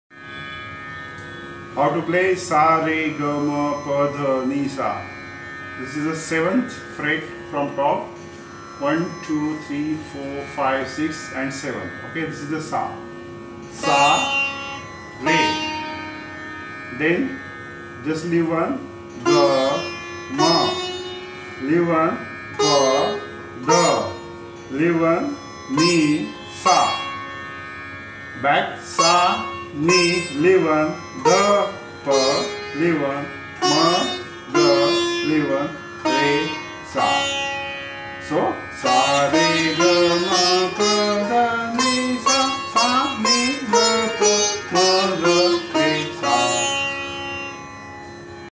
Sitar Lesson # 0 - How to play Sa Re Ga Ma on Sitar
In this video you can learn how to play Sa Re Ga Ma on Sitar.